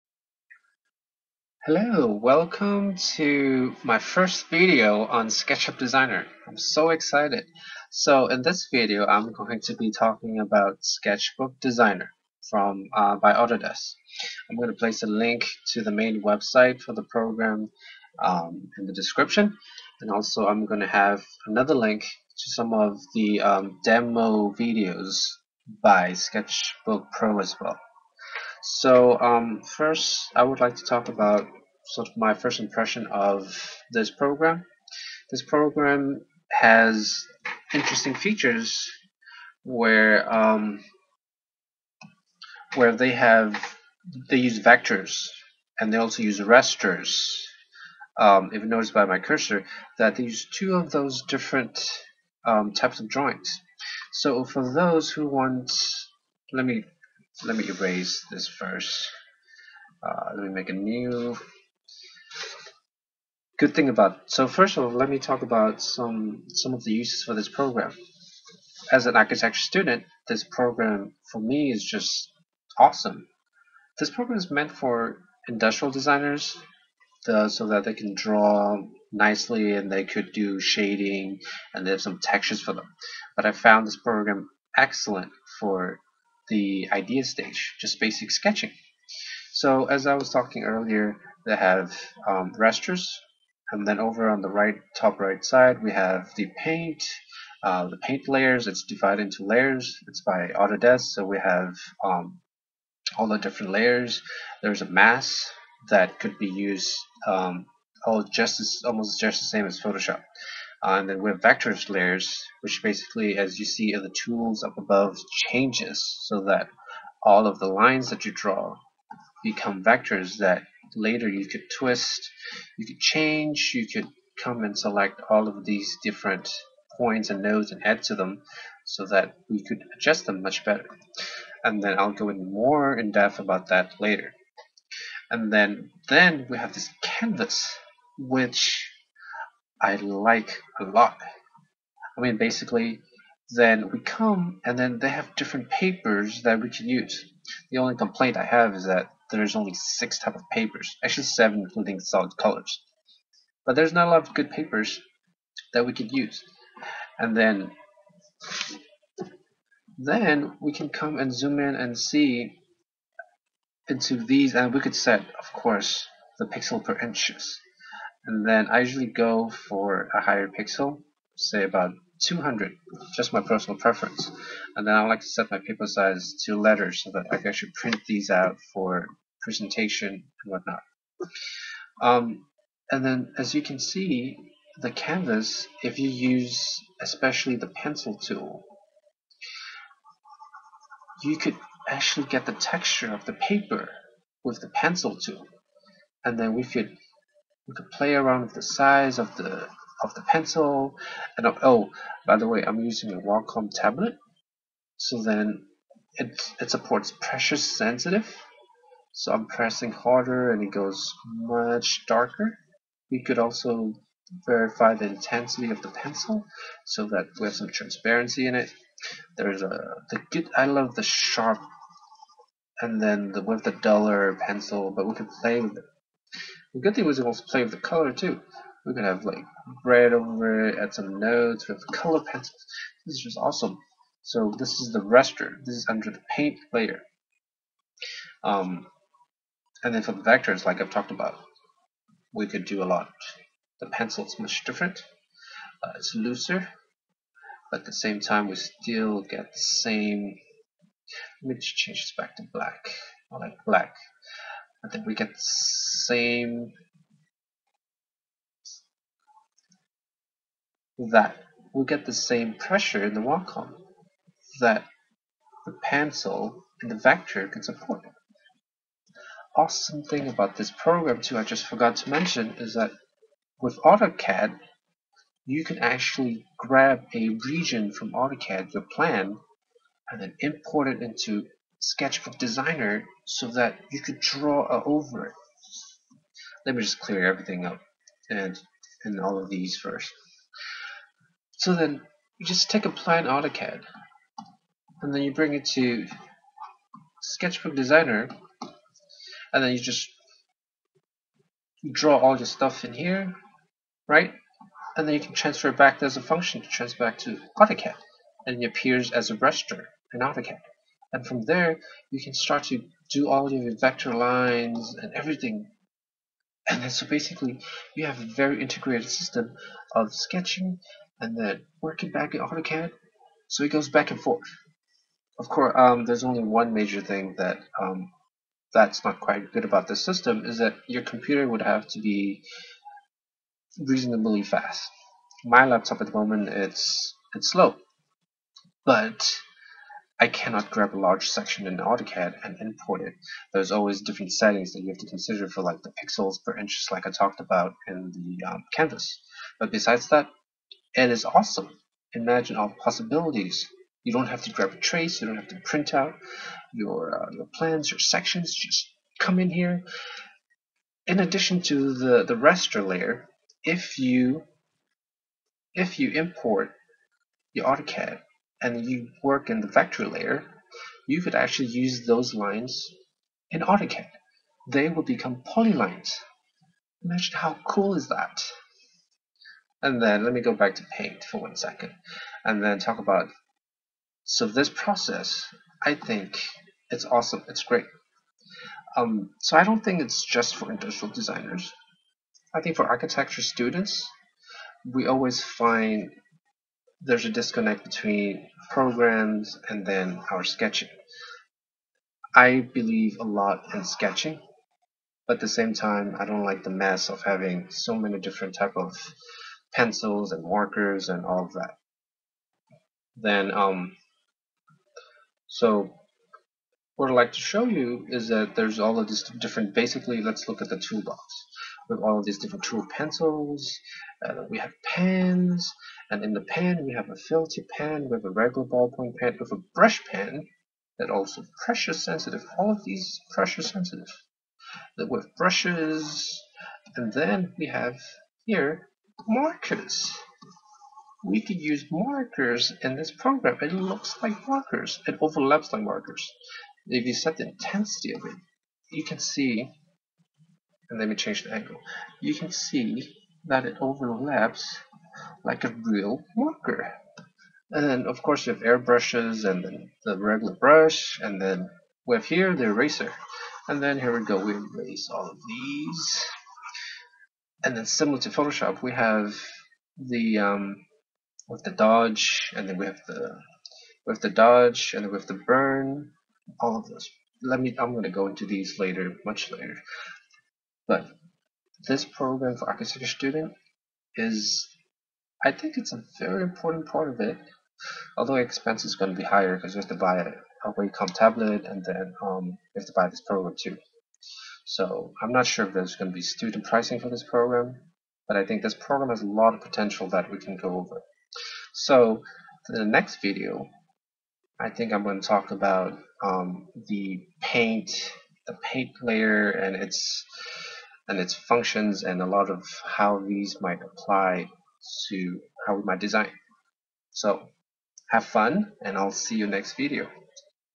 Sketchbook Designer Intro and review- Part 1.avi
This fist part intro is on the uses of sketchbook designer in architectural studio. We can use this to bridge the gap between sketching and cad work. See the links below for the main website for the program and a basic demo playlist as well from the Sketchbook Pro channel  (a sister program).